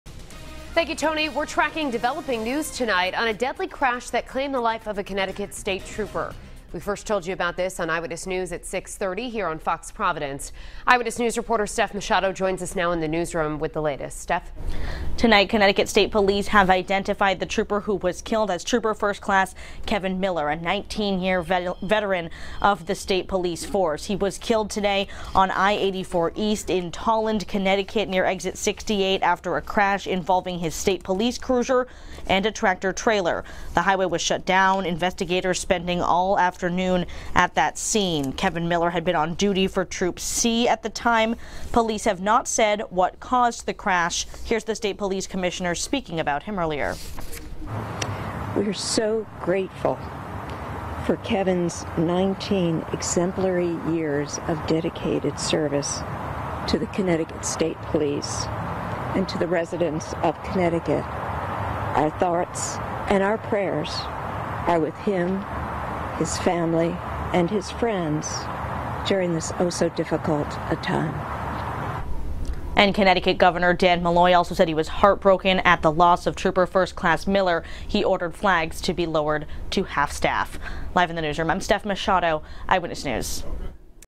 Connecticut trooper killed in crash with tractor-trailer
A veteran Connecticut state trooper has been killed in a highway crash involving a tractor-trailer.